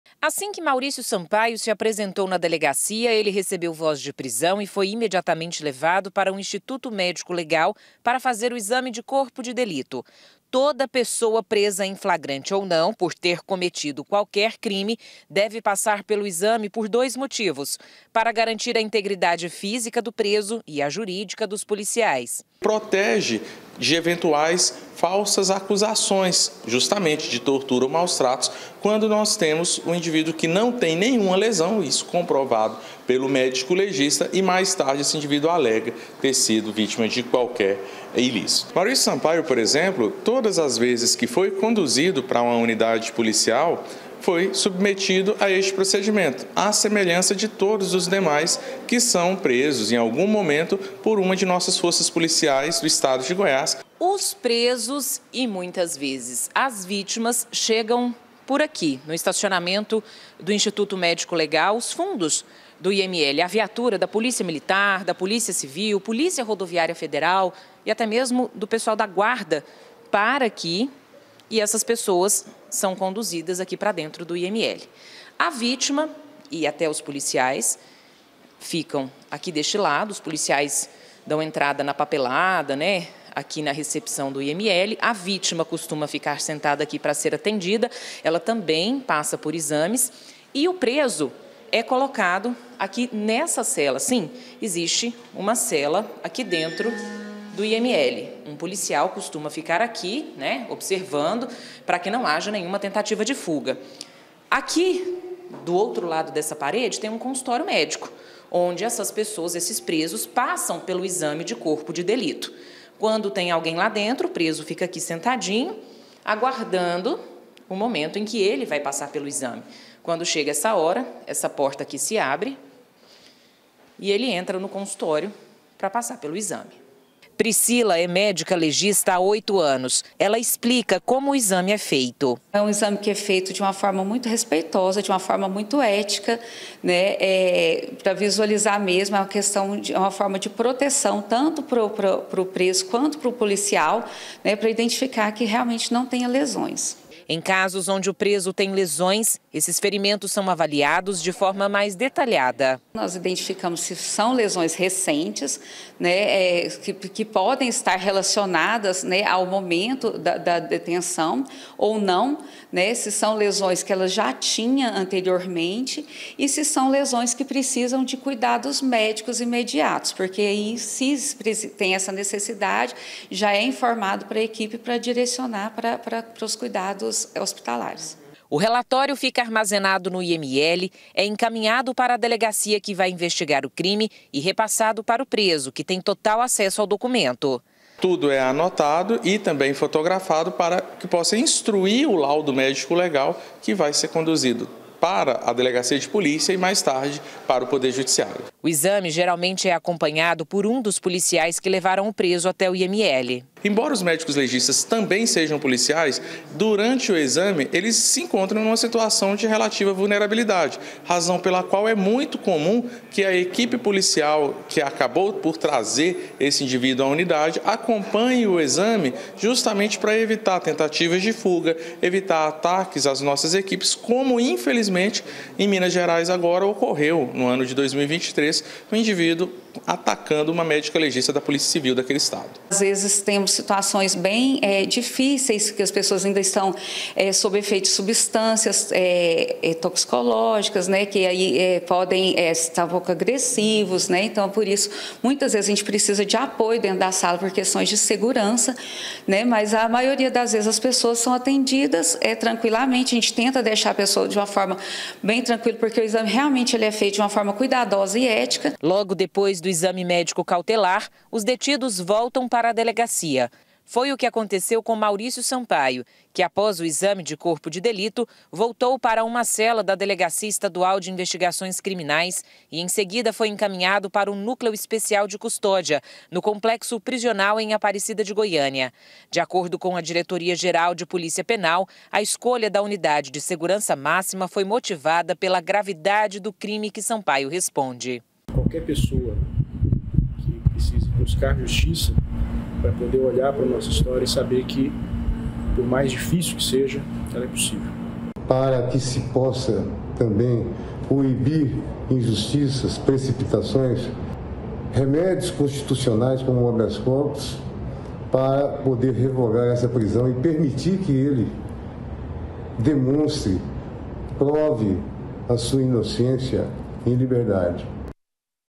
CASO VALÉRIO LUIZ: MAURÍCIO SAMPAIO CUMPRE PENA NO COMPLEXO PRISIONAL
JÁ CUMPRE PENA NO COMPLEXO PRISIONAL DE APARECIDA O EMPRESÁRIO MAURÍCIO SAMPAIO, CONDENADO POR SER O  MANDANTE DA MORTE DO RADIALISTA VALÉRIO LUIZ, EM 2012. MAURÍCIO SE ENTREGOU ONTEM À POLÍCIA E, COMO TODO PRESO, PASSOU POR EXAME DE CORPO DE DELITO. A NATHÁLIA MENDONÇA MOSTRA COMO FUNCIONA ESSE PROCEDIMENTO, QUE É IMPORTANTE TANTO PARA A PROTEÇÃO DO PRESO COMO PARA A DOS POLICIAIS.